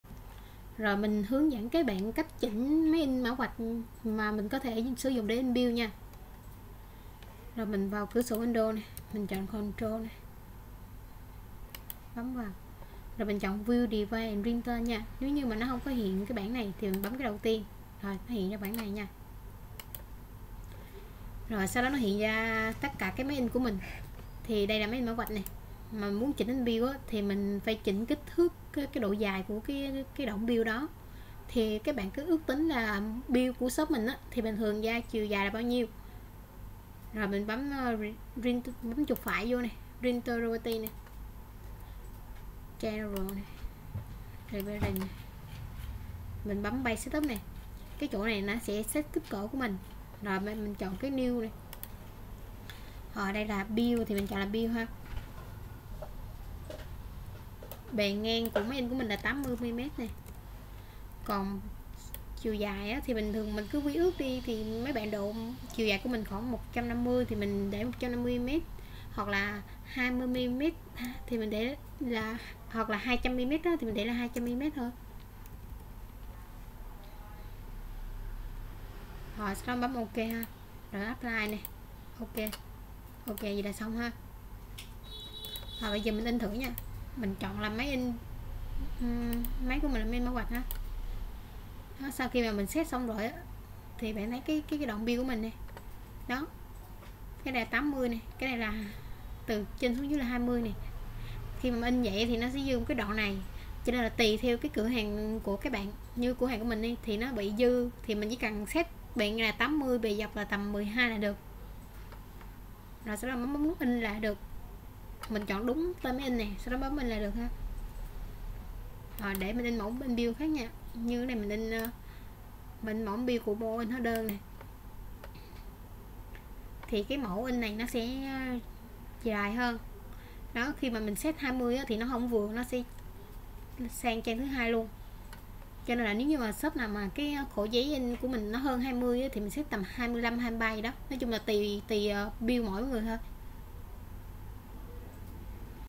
Hướng dẫn setup máy in 2 trong 1 Xprinter 350B (in tem kiêm in hóa đơn)
- Ước chừng bill ở shop mỗi tờ khoảng bao nhiêu cm, set kích thước giấy in cỡ đso trong driver là được.
- Không cắt tự động được, nếu shop nào mà hoá đơn dài ngắn ko đều thì nó sẽ bị thiếu hoặc dư nhiều.